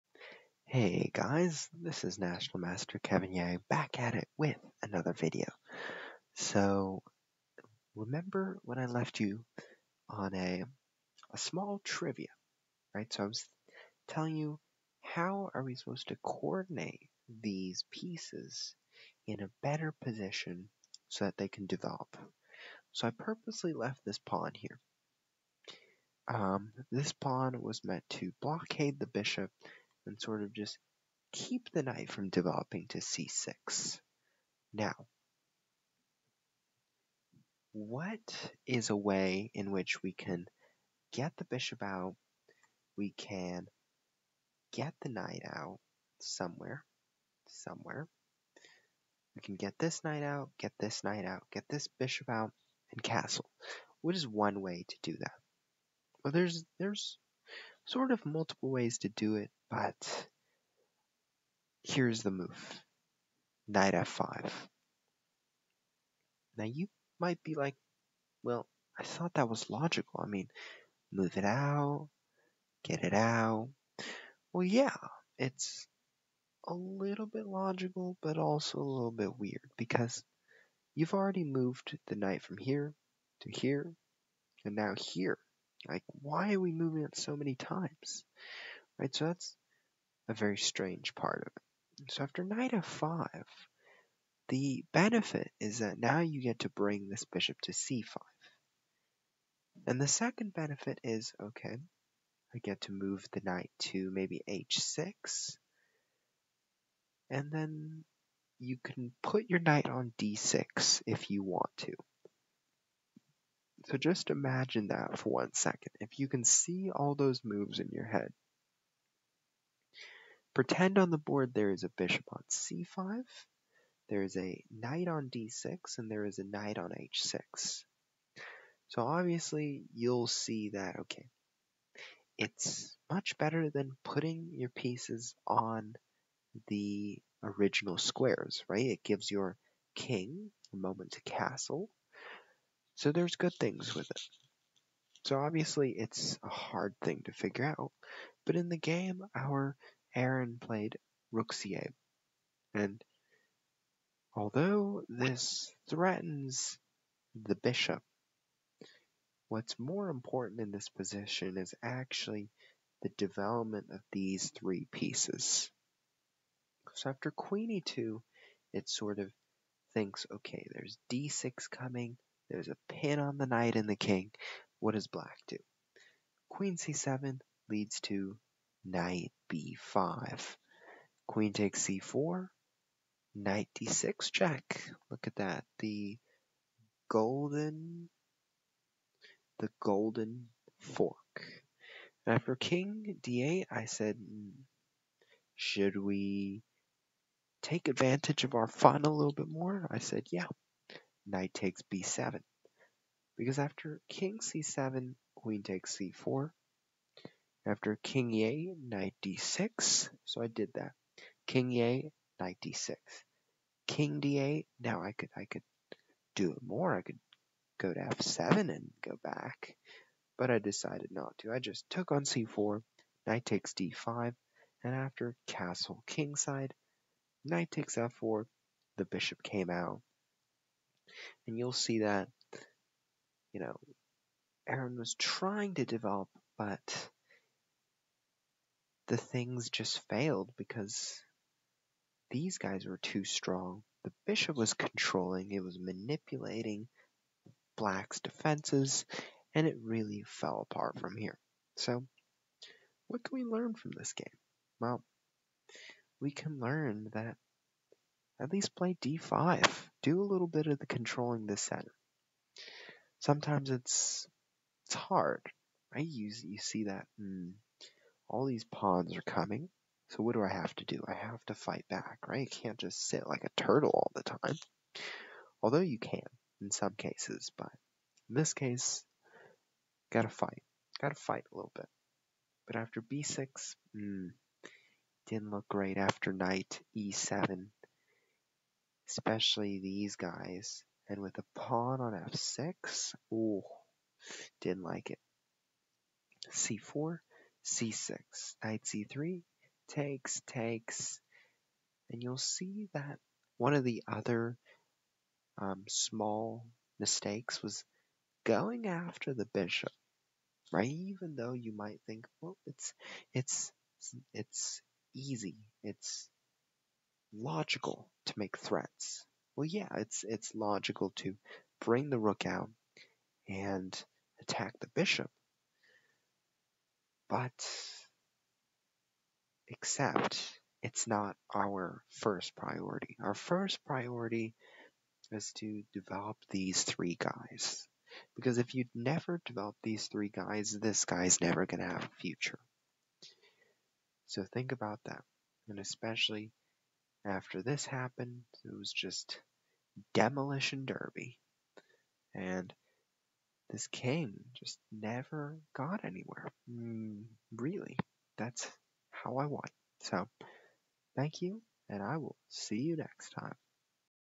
More About the Bird's Defense With Aron
Here within this video, I talk about how one should prioritize development over making simple threats and recognize the danger that the center poses. I also make a point to demonstrating how the pieces should comfortably be placed and give a small picture on how each of the pieces coordinate with one another. Even with a bad position, development is still necessary!